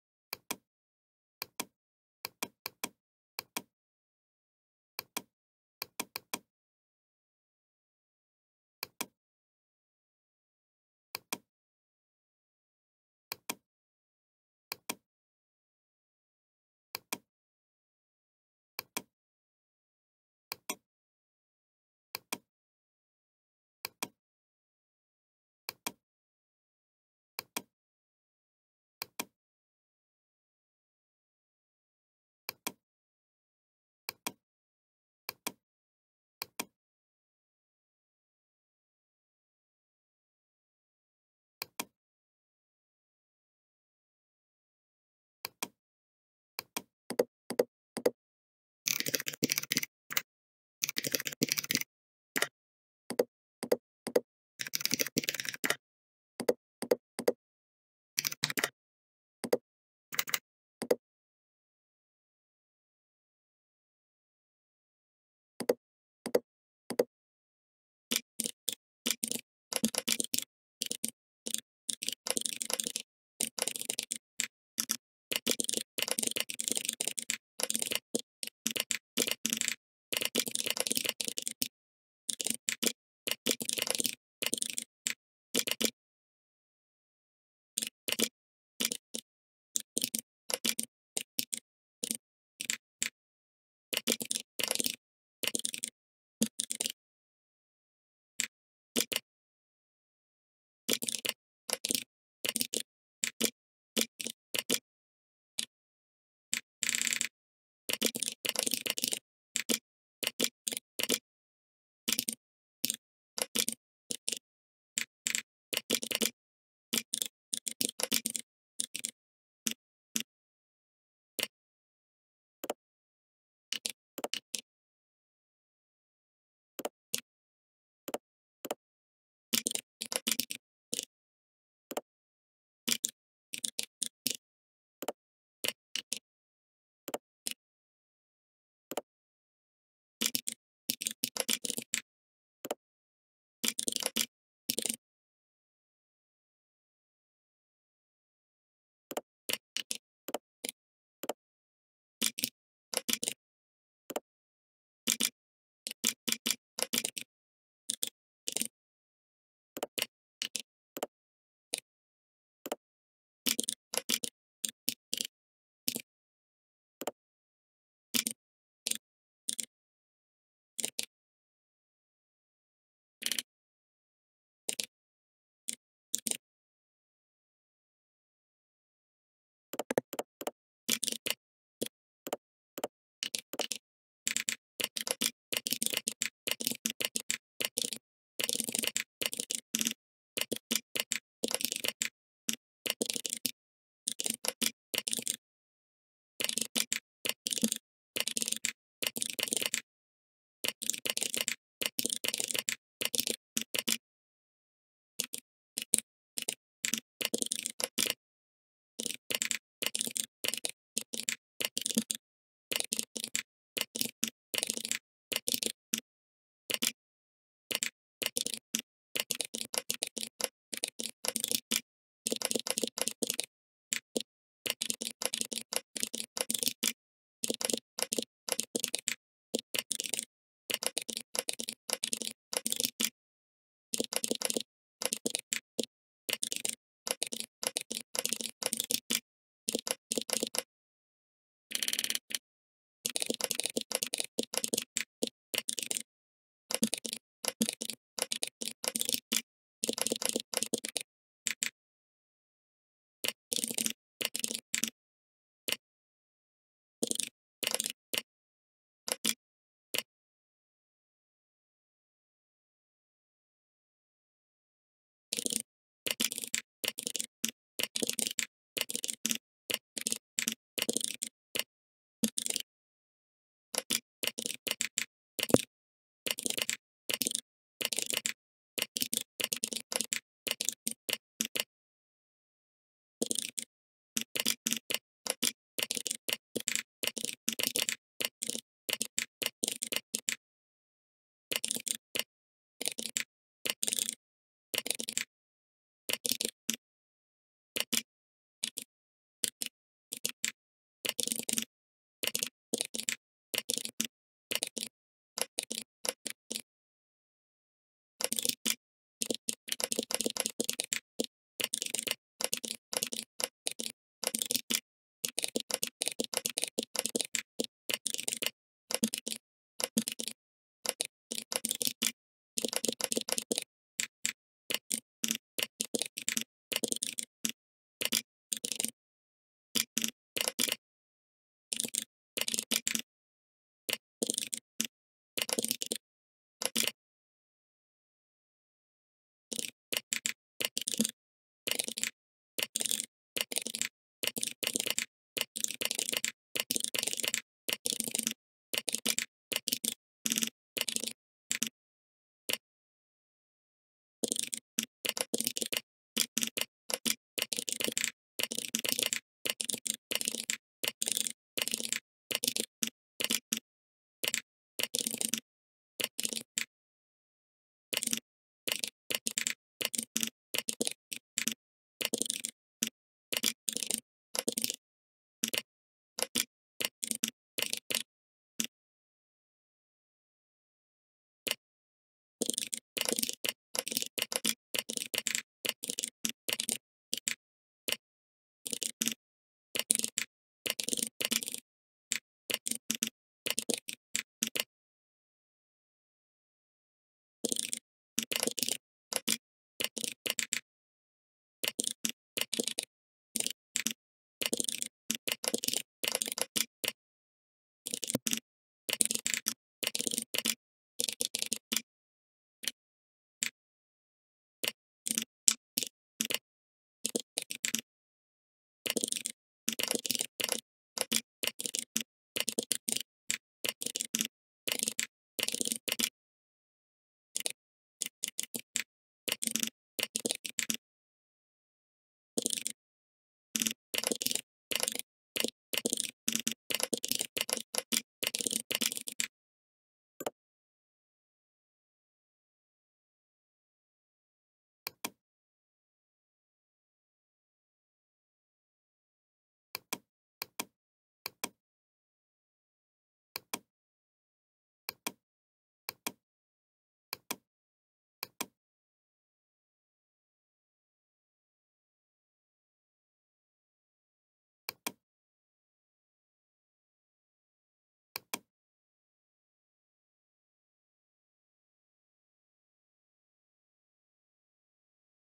ASMR Programming - Claymorphism List Hover Effect Using HTML CSS - No Talking
Hello Everyone ✨ Explore the fascinating world of Claymorphism List Hover Effects in this step-by-step tutorial. Discover how to use HTML and CSS to create captivating interactive list elements that mimic the soft and tactile appearance of clay. Learn the techniques behind applying subtle transitions, shadows, and hover effects to bring your lists to life. Perfect for web designers and developers looking to add a unique and engaging touch to their websites. Join us as we delve into the art of Claymorphism and unlock the secrets of creating stunning list hover effects using HTML and CSS.

Video Contents :

00:00 Preview
00:47 Creat files
00:57 HTML Coding
03:00 CSS Coding
07:29 Final Result

Subscriptions, good comments and likes are great support for me 😍
All copyrights by ProgProd